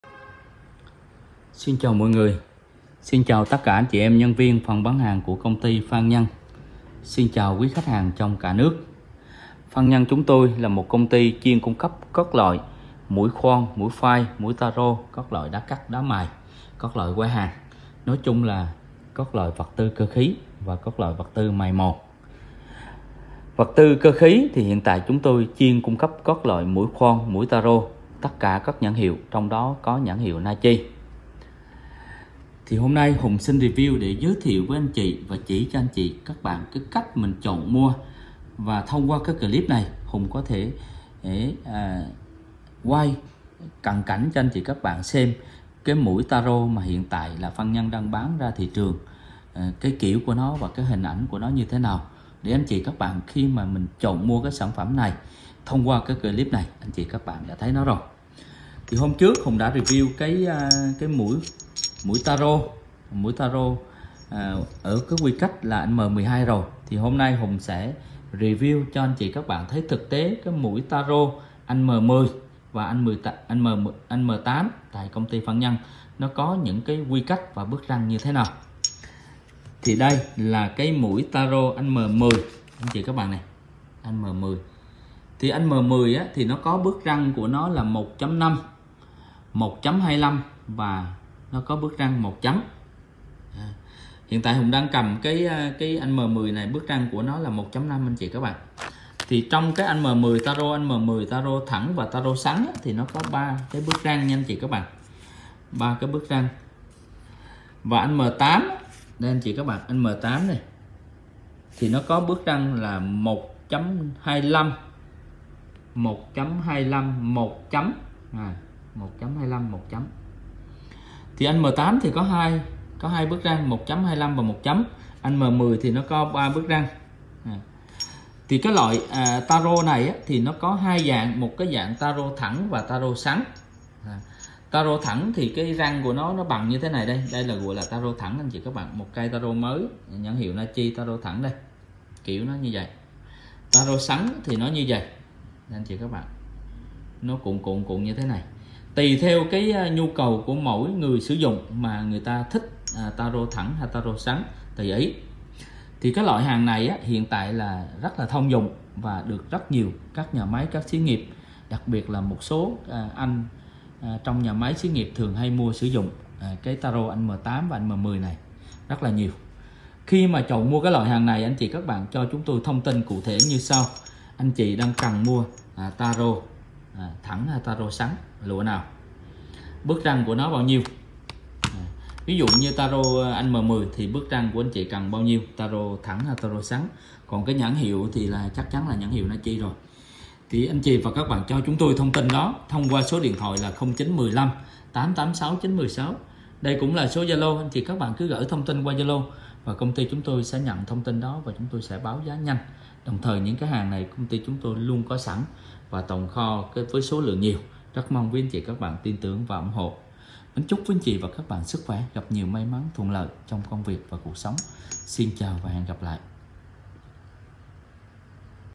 Mũi Taro thẳng , Taro xoắn M8 - M10 - Công ty Phan Nhân
Mũi taro rãnh thẳng Nachi - Nhật là dụng cụ dùng để tạo ren phổ biến nhất, nó dùng để tạo ren lỗ bên trong hoặc tạo ren bên ngoài (gọi là mũi taro ren ngoài hoặc bàn ren).Taro ren có 2 loại ( Taro thẳng và Taro xoắn ) hiệu Nachi Nhật
Mũi taro thẳng Nachi là loại taro máy, nó có thể dùng cho các máy phay CNC, máy tiện CNC, máy phay, máy khoan taro, các máy taro chuyên dụng, máy khoan từ...
- Mũi taro thẳng Nachi chỉ có 1 cây, Mũi taro rãnh thẳng Nachi – Nhật dùng để gia công lỗ kín khi cắt sẽ móc phoi lên trên. Như vậy thì mũi taro thẳng Nachi có thể gia công được cả lỗ bít và lỗ thông.